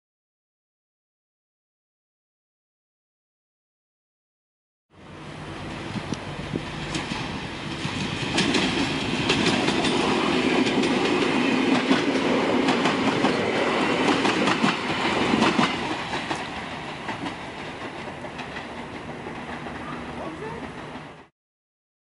EN57-063 [ PR ] Oborniki Wlkp D29-354 #20150217
osobowy do Kołobrzegu mija przejazd na ulicy Kowanowskiej